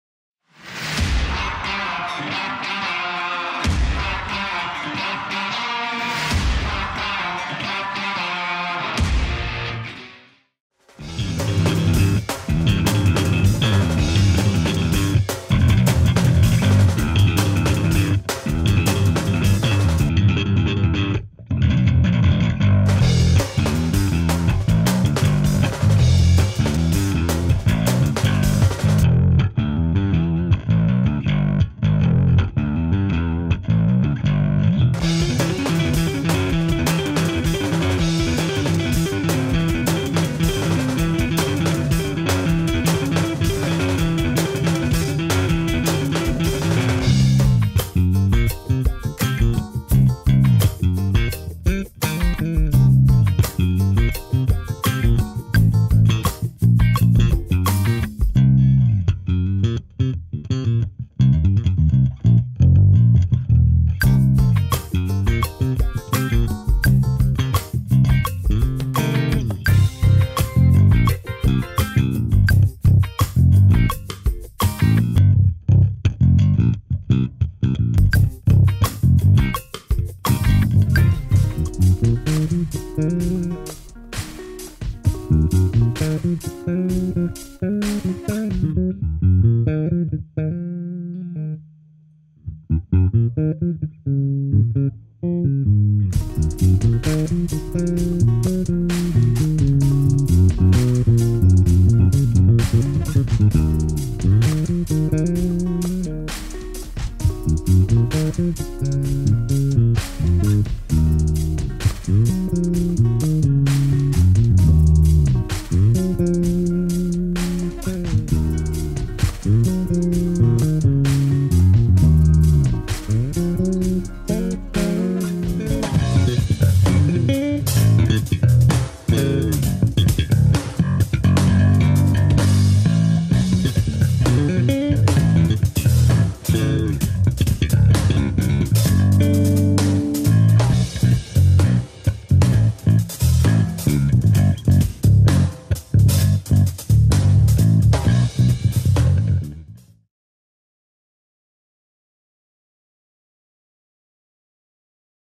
Gallien-Krueger Fusion 210 Bass Combo Review
Bass Gear Magazine's Sean Fairchild reviews the Gallien-Krueger (GK) Fusion 210 bass combo over This is the companion video for that review.